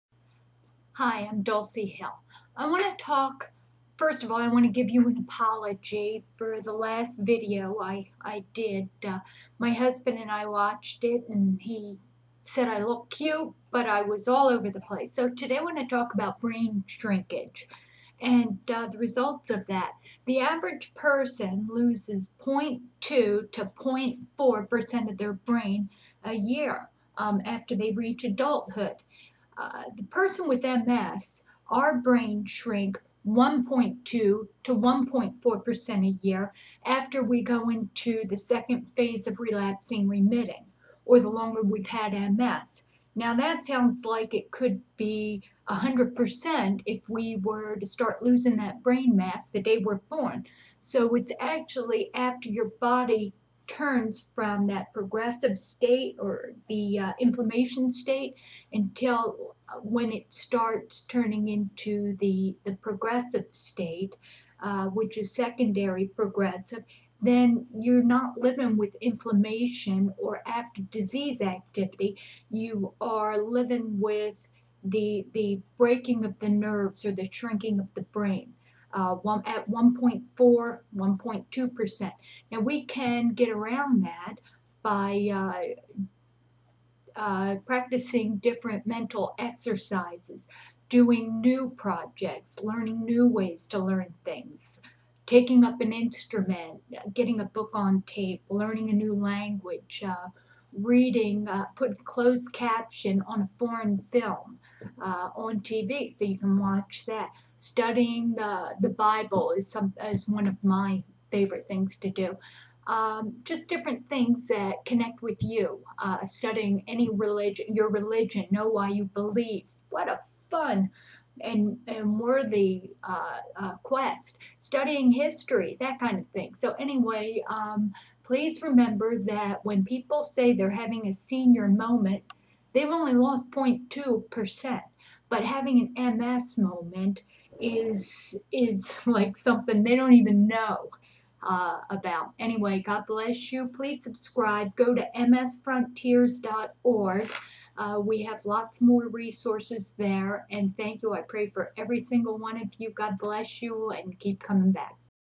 Brain shrinkage in MS
The Brain of a normal person begins shrinking in adulthood at a rate of .2-.4% a year which results in cognitive difficulties or senior moments. A person in the secondary phase of MS or with progressive MS looses between 1.2% and 1.4% per year resulting in premature senior moments.  By the way, Taking Alpha Lipoic Acid has been shown to be effective is slowing brain loss.  Please search for the video on this. For those who like the longer version of this